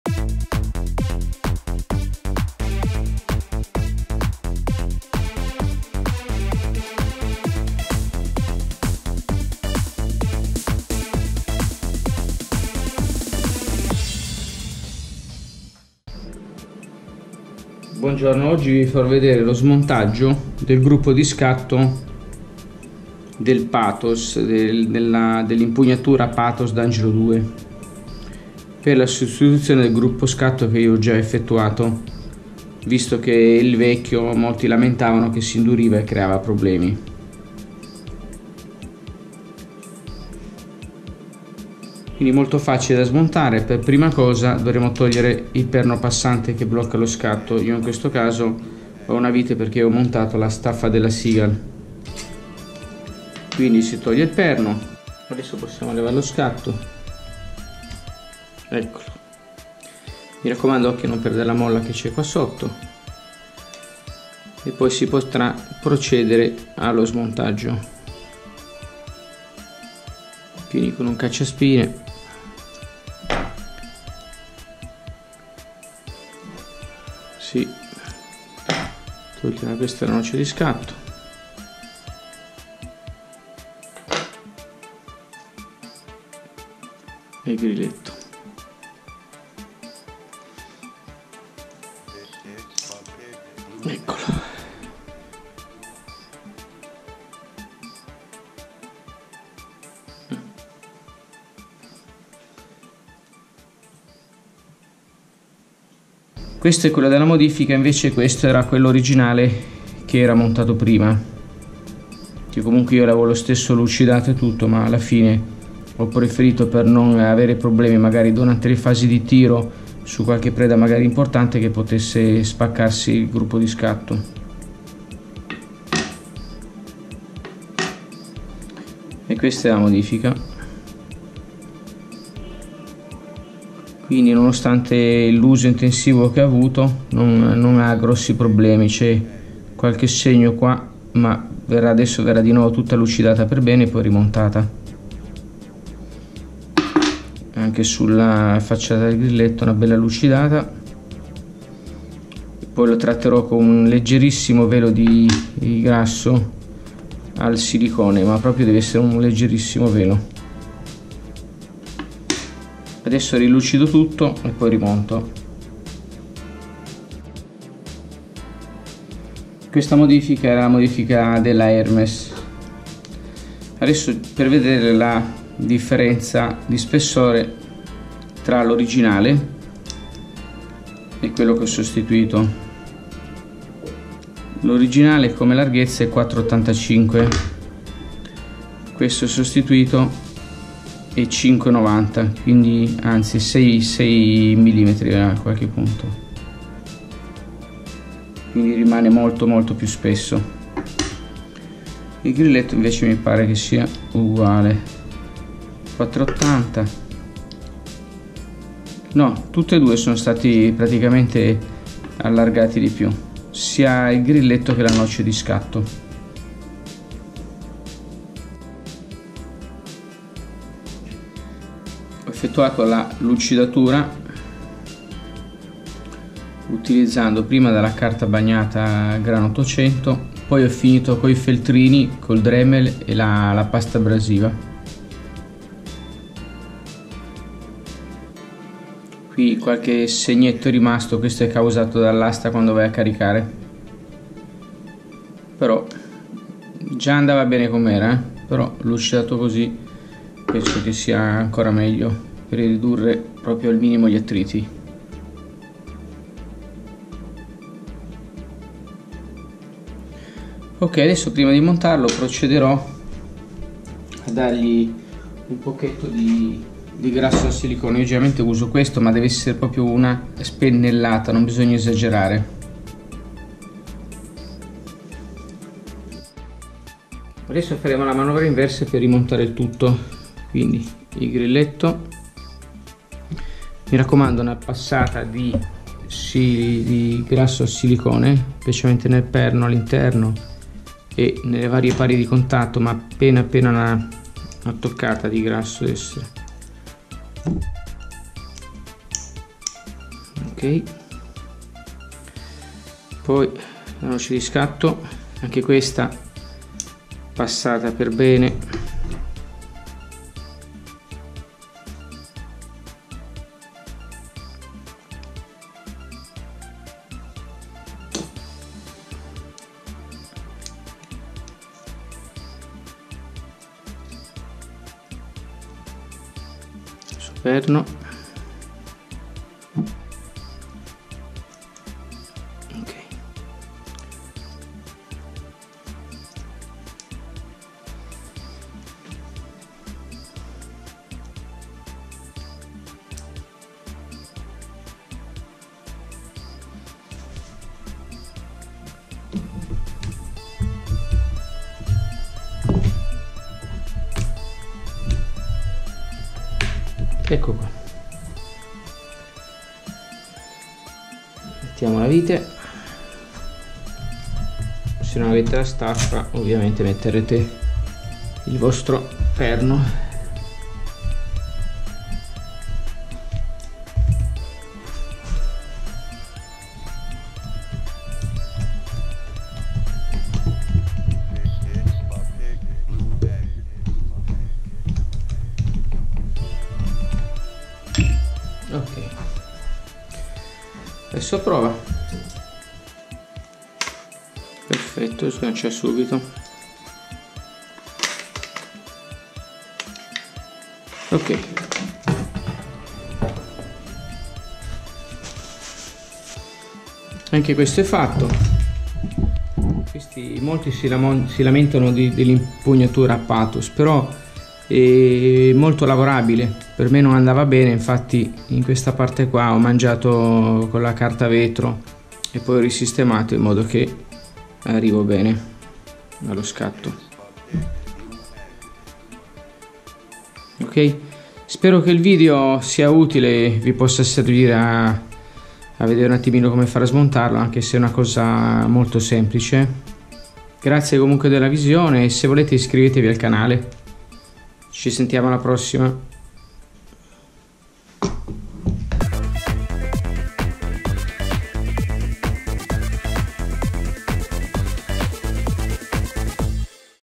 Modifica scatto Pathos D'Angelo II con Kit Ermes
Breve video per la sostituzione dello scatto con il Kit Ermes. Io l'avevo già montato e ho voluto smontarlo per vedere le sue condizioni. Ho ridato una bella lucidata al grilletto e alla noce dello scatto e rimontato il tutto. In un secondo tempo ho rimesso due rondella, fatte in delrin, da 8 mm di diametro con foro da 5 mm e spessore 0,50 mm, nel perno dello scatto perchè così rimane più centrato,